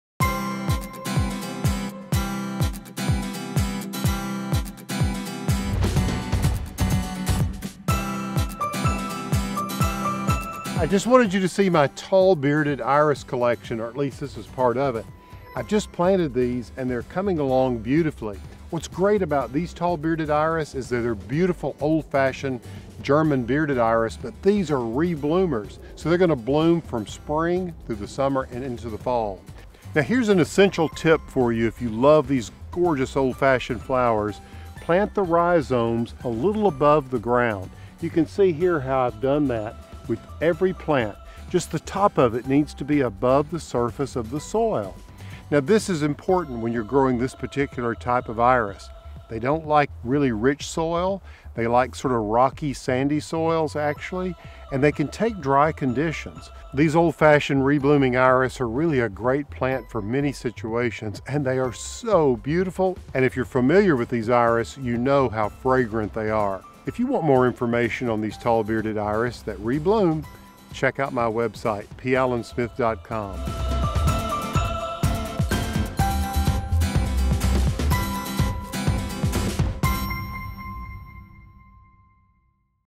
Growing Tall Bearded Iris: Re-Blooming Perennial
Garden Home host P. Allen Smith shows how to grow re-blooming Iris.

Topics Include:
Tall Bearded Iris
How to Plant Iris
Old Fashioned Perennials

"Bloom with Us" is a partnership with First Community Bank and P. Allen Smith to design and install pollinator gardens in an effort to raise awareness of the benefits of pollinators.